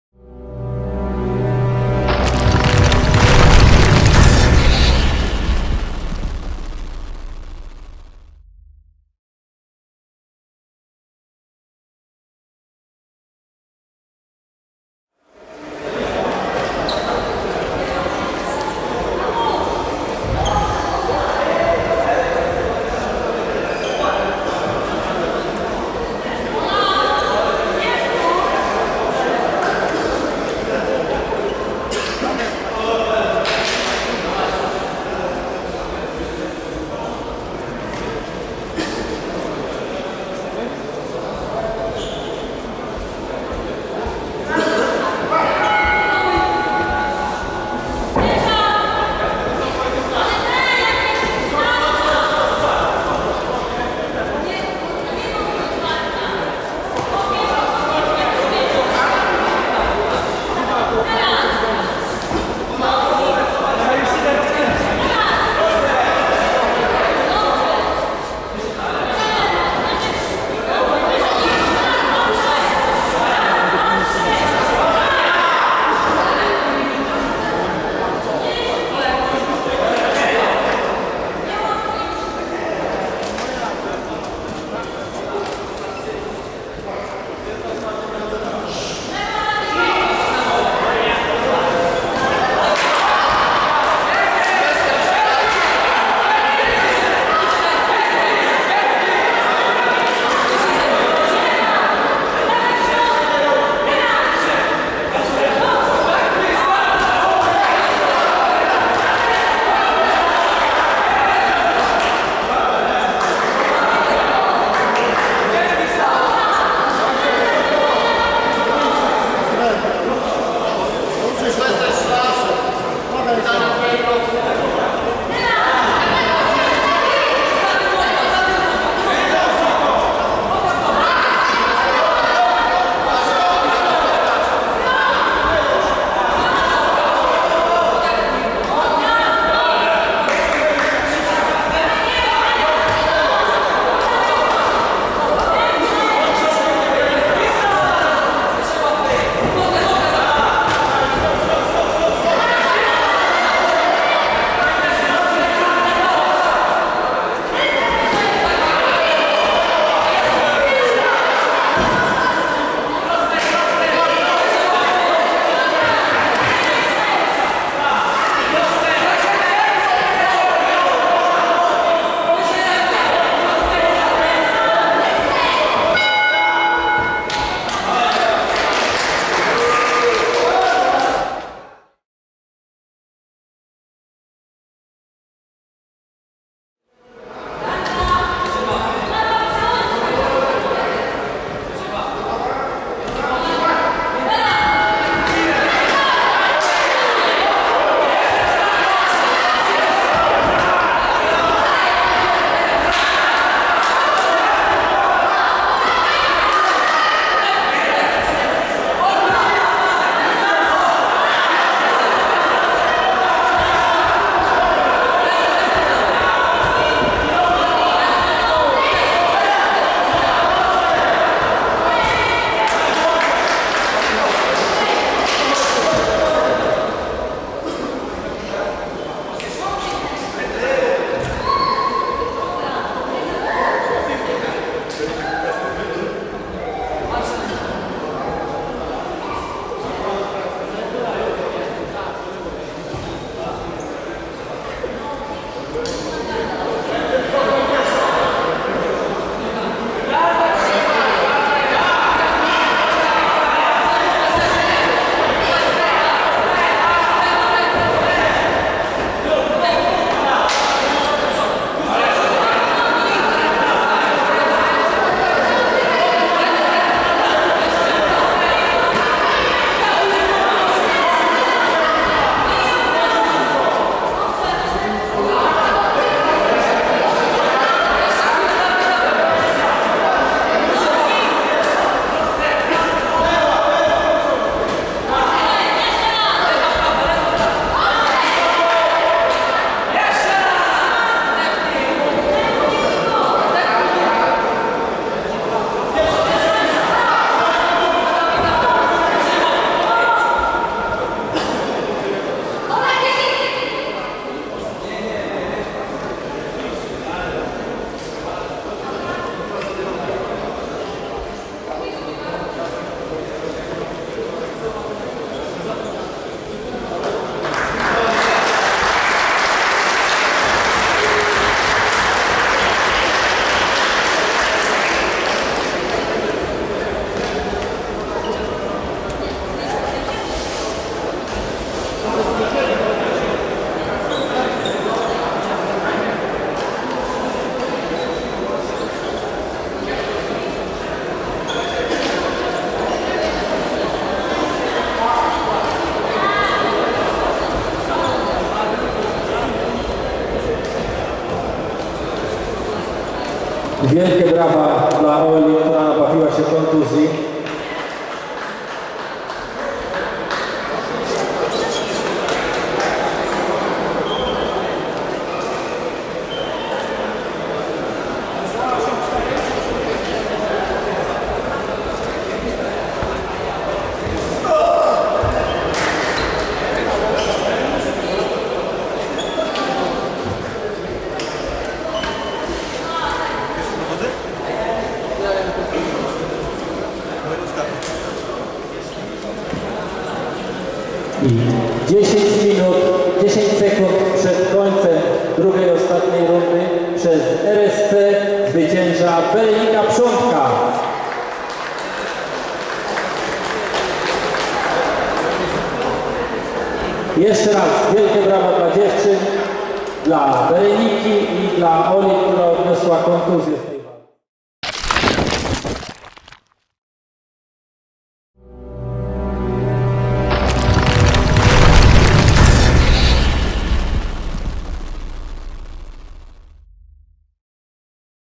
MLS 38 - 67 kg: Berenika Prządka (Fight Academy Stargard) vs. Ola Milczarek (TNT Kobyłka)
MLS 38, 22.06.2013, Płońsk, low-kick, 67 kg, Berenika Prządka, Fight Academy Stargard, vs., Ola Milczarek, TNT Kobyłka,

MMLS 2013 - współfinansowana jest z budżetu Urzędu Marszałkowskiego Województwa Mazowieckiego